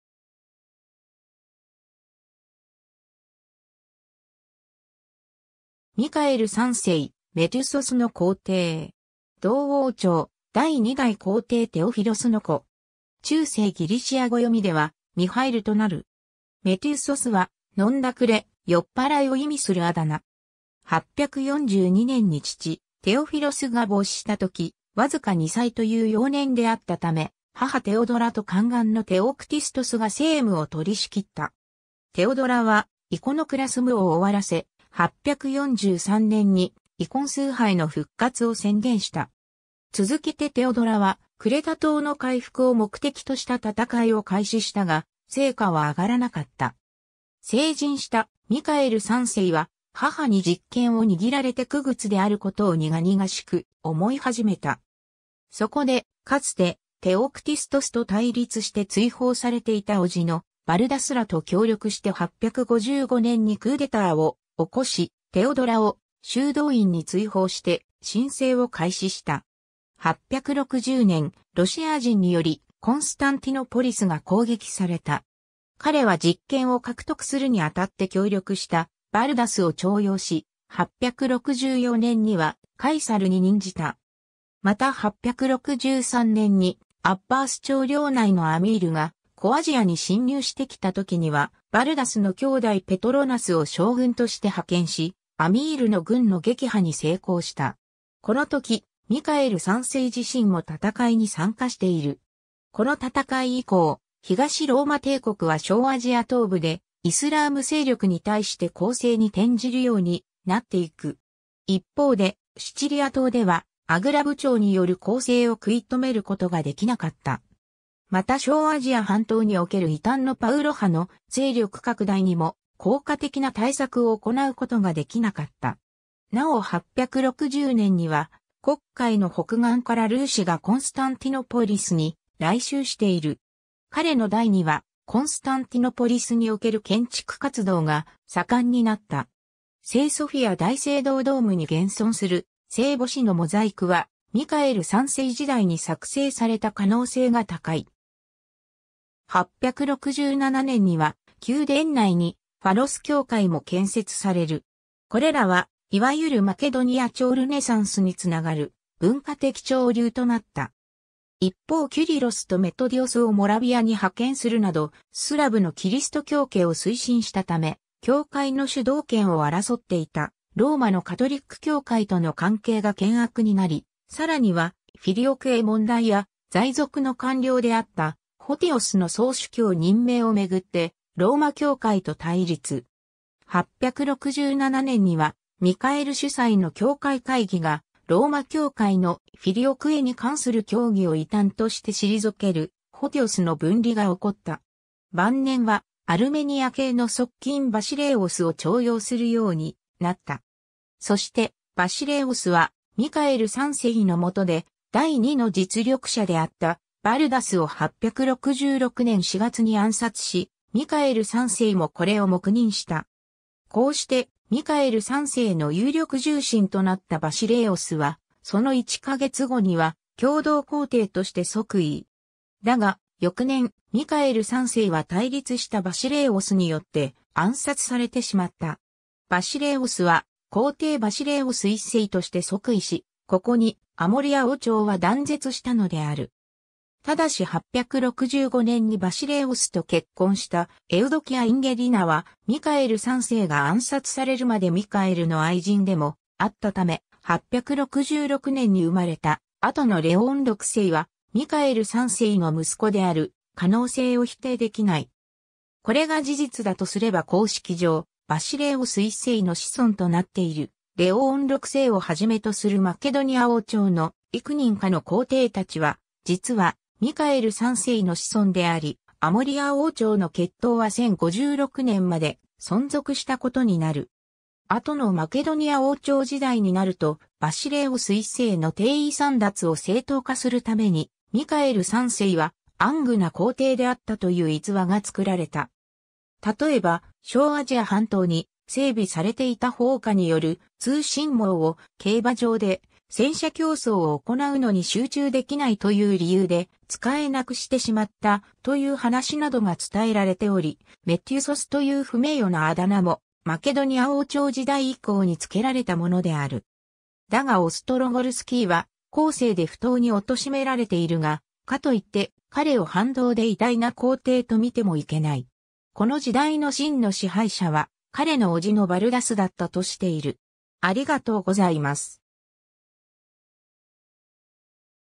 ミカエル3世
ミカエル3世“メテュソス”（ギリシア語：Μιχαήλ Γʹ ὁ Μέθυσος, Mikhaēl III ho Methysos, 840年1月19日または20日 - 867年9月23日または24日は、東ローマ帝国アモリア王朝の第3代（最後）の皇帝（在位：842年 - 867年）。
同王朝第2代皇帝テオフィロスの子。
中世ギリシア語読みでは「ミハイル」となる。
“メテュソス”は「飲んだくれ」「酔っ払い」を意味するあだ名。
 842年に父テオフィロスが没したとき、わずか2歳という幼年であったため、母テオドラと宦官のテオクティストスが政務を取り仕切った。
テオドラはイコノクラスムを終わらせ、843年にイコン崇拝の復活を宣言した。
続けてテオドラはクレタ島の回復を目的とした戦いを開始したが、成果は上がらなかった。
 成人したミカエル3世は、母に実権を握られて傀儡であることを苦々しく思い始めた。
そこで、かつてテオクティストスと対立して追放されていた叔父のバルダス（テオドラの弟。中世ギリシャ語読みでは「ヴァルダス」）らと協力して855年にクーデターを起こし、テオドラを修道院に追放して親政を開始した。
 860年、ロシア人によりコンスタンティノポリスが攻撃された（en）。
 彼は実権を獲得するにあたって協力したバルダスを重用し、864年には「カイサル」（副皇帝）に任じた。
また863年にアッバース朝領内のアミールが小アジアに侵入してきた時にはバルダスの兄弟ペトロナスを将軍として派遣し、アミールの軍の撃破に成功した。
この時ミカエル3世自身も戦いに参加している。
この戦い以降、東ローマ帝国は小アジア東部でイスラーム勢力に対して攻勢に転じるようになっていく。
一方でシチリア島では、アグラブ朝による攻勢を食い止めることができなかった。
また小アジア半島における異端のパウロ派の勢力拡大にも、効果的な対策を行うことができなかった。
なお860年には黒海の北岸からルーシ（のちのキエフ・ルーシ）がコンスタンティノポリスに来襲している。
 彼の代にはコンスタンティノポリスにおける建築活動が盛んになった。
聖ソフィア大聖堂ドームに現存する聖母子のモザイクはミカエル3世時代に作成された可能性が高い。
867年には宮殿内にファロス教会も建設される。
これらはいわゆるマケドニア朝ルネサンスにつながる文化的潮流となった。
一方キュリロスとメトディオスをモラヴィアに派遣するなど、スラヴのキリスト教化を推進したため、教会の主導権を争っていたローマのカトリック教会との関係が険悪になり、さらには「フィリオクェ問題」や在俗の官僚であったフォティオスの総主教任命をめぐってローマ教会と対立。
867年にはミカエル主宰の教会会議がローマ教会の「フィリオクエ」に関する教義を異端として退ける「フォティオスの分離」が起こった。
 晩年は、アルメニア系の側近バシレイオスを重用するようになった。
そしてバシレイオスは、ミカエル3世のもとで第2の実力者であったバルダスを866年4月に暗殺し、ミカエル3世もこれを黙認した。
こうしてミカエル3世の有力重臣となったバシレイオスは、その1ヵ月後には共同皇帝として即位。
だが翌年、ミカエル3世は対立したバシレイオスによって暗殺されてしまった。
バシレイオスは皇帝バシレイオス1世として即位し（マケドニア王朝初代皇帝）、ここにアモリア王朝は断絶したのである。
ただし865年にバシレイオスと結婚したエウドキア・インゲリナは、ミカエル3世が暗殺されるまでミカエルの愛人でもあったため、866年に生まれた、後のレオーン6世はミカエル3世の息子である可能性を否定できない。
これが事実だとすれば公式上、バシレイオス1世の子孫となっているレオーン6世をはじめとするマケドニア王朝の幾人かの皇帝達は実はミカエル3世の子孫であり、アモリア王朝の血統は1056年まで存続したことになる。
 後のマケドニア王朝時代になると、バシレイオス1世の帝位簒奪を正当化するために、ミカエル3世は暗愚な皇帝であったという逸話が作られた。
例えば、小アジア半島に整備されていた烽火による通信網を「競馬場で戦車競走を行うのに集中できない」という理由で使えなくしてしまった、という話などが伝えられており、“メテュソス”という不名誉なあだ名もマケドニア王朝時代以降につけられたものである。
だがオストロゴルスキーは「後世で...